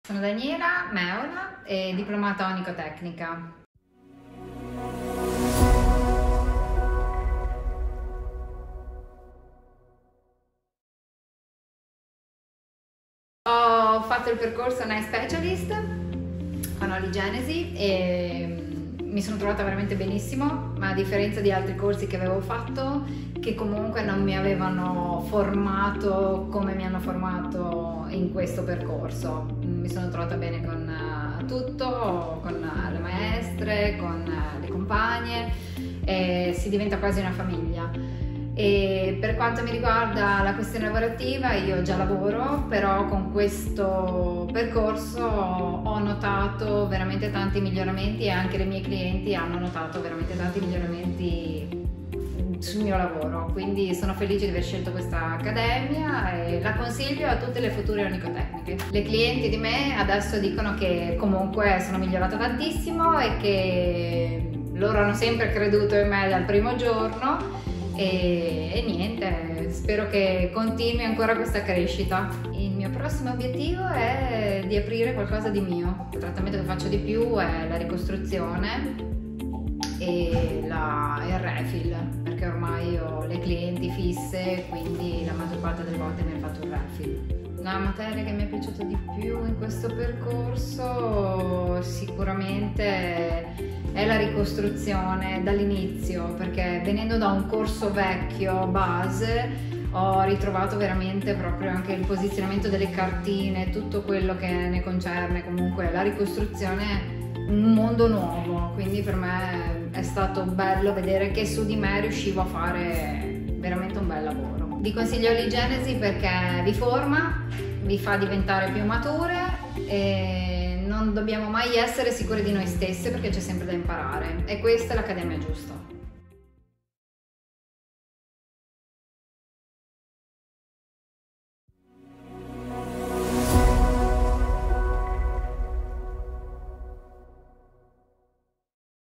Testimonianza di Daniela Meola sul Percorso Nail Specialist
Guarda la testimonianza di Daniela Meola dopo il completamento del Percorso Professional Academy Nail Specialist.